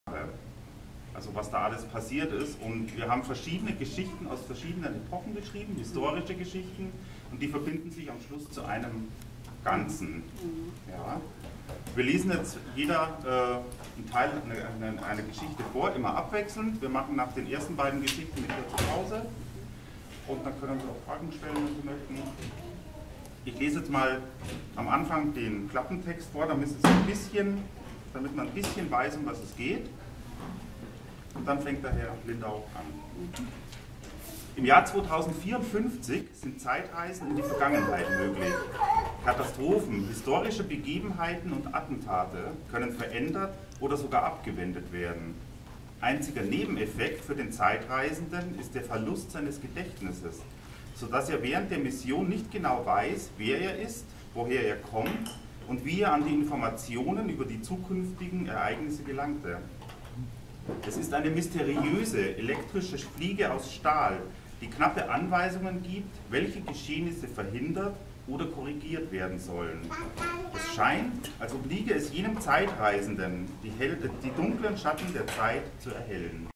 Wolfgang Brunner liest aus "Die hellen Schatten der Zeit", 27.06.2015 Mayersche Buchhandlung Wesel
Wolfgang Brunner liest aus "Die hellen Schatten der Zeit"

Am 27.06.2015 ion der Mayersche Buchhandlung Wesel, zusammen  mit Co-Autor Rolf Lindau

# Taschenbuch: 444 Seiten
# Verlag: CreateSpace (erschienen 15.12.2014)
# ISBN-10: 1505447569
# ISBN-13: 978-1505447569
# EUR 14,99

Im Jahr 2054 sind Zeitreisen in die Vergangenheit möglich.
Katastrophen, historische Begebenheiten und Attentate können verändert oder sogar abgewendet werden. Einziger Nebeneffekt für den Zeitreisenden ist der Verlust seines Gedächtnisses, so dass er während der Mission nicht genau weiß, wer er ist, woher er kommt und wie er an die Informationen über die zukünftigen Ereignisse gelangte.
Es isteine mysteriöse, elektrische Fliege aus Stahl, die knappe Anweisungen gibt, welche Geschehnisse verhindert oder korrigiert werden sollen. Es scheint als obliege es jenem Zeitreisenden, die dunklen Schatten der Zeit zu erhellen...